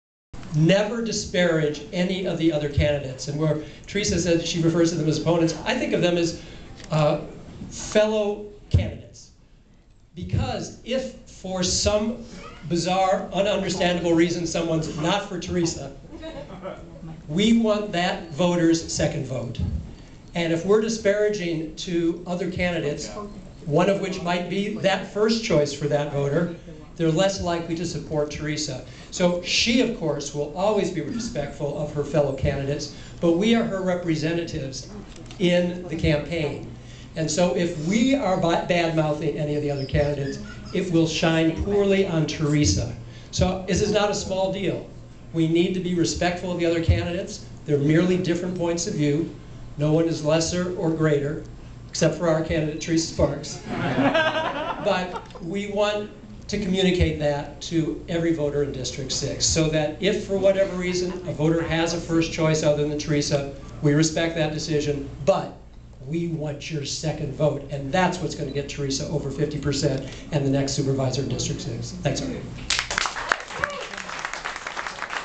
7-31-10MarkLenoRankChoiceVoting.MOV
7-31-10 State Senator Mark Leno on rank  choice voting at District 6 Supervisor Theresa Sparks Candidate Campaign Kick-off.